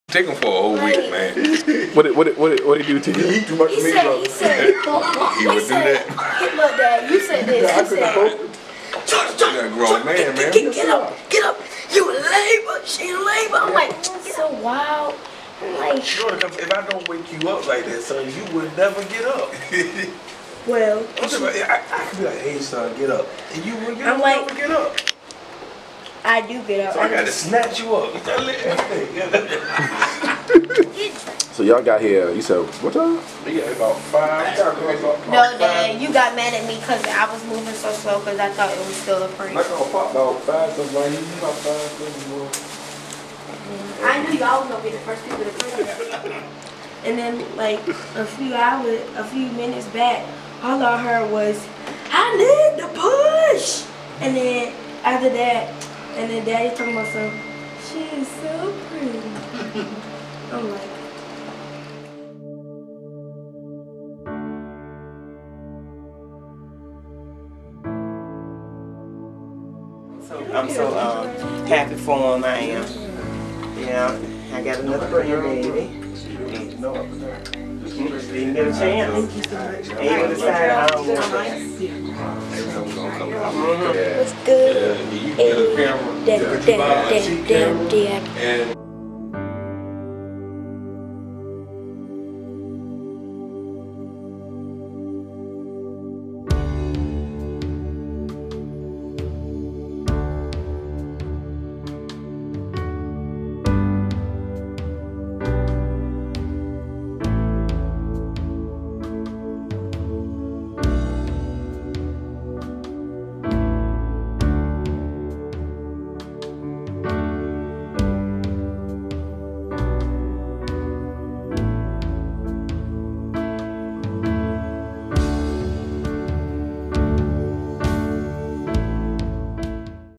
The Day Princess Ava Powell was born!
What a precious moment. Help me congratulate Tavaris and Lanita on the birth of their beautiful princess Ava Powell.